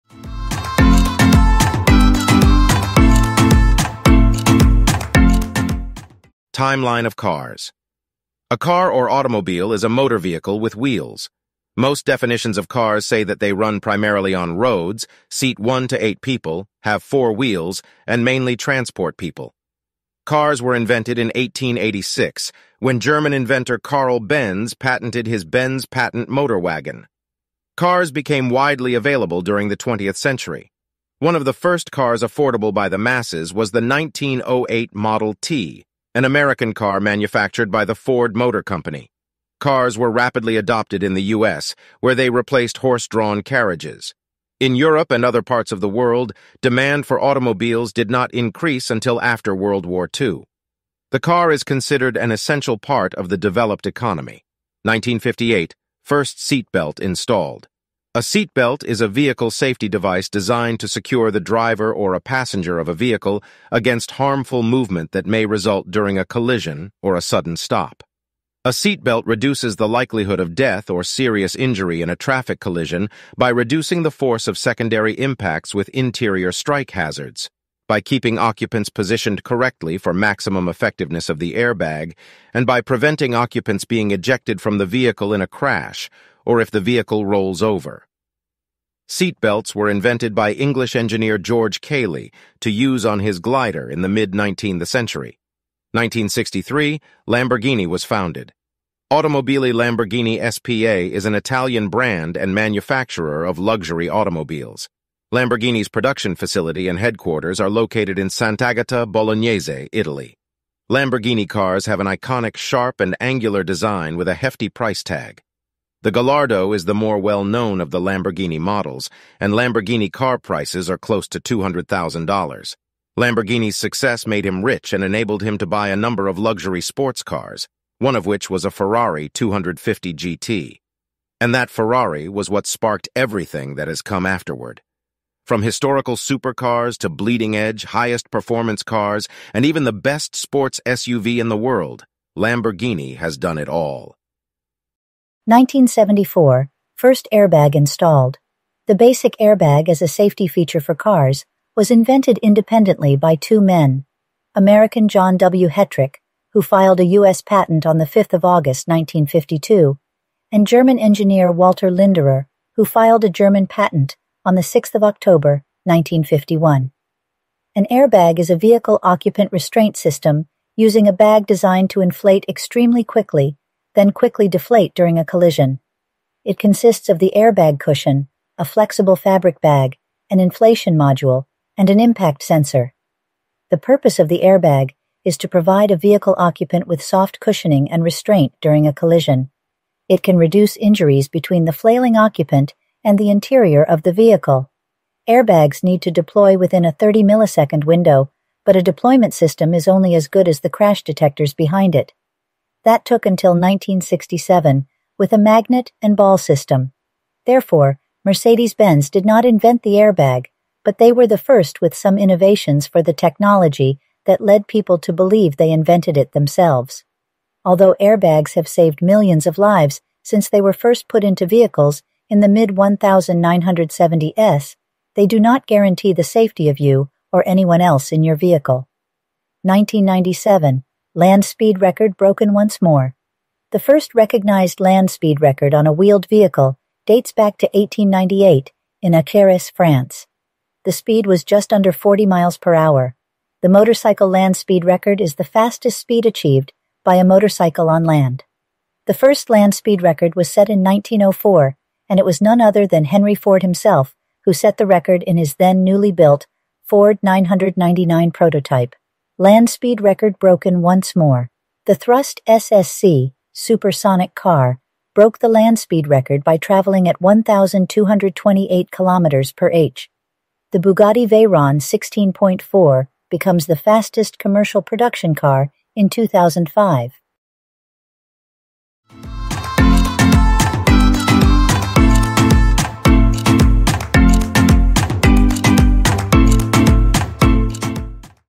History of Cars and Car Safety Innovations
A timeline of significant events in the history of cars and automobiles. From the invention of the car in 1886 to the installation of seat belts in 1958 and the founding of Lamborghini in 1963.
This video clip provides an overview of the history of cars and their widespread adoption as an essential part of the developed economy. It also highlights some of the key safety innovations that have been introduced in cars over the years, including the seatbelt, airbag, and more.

✅ Many thanks giving you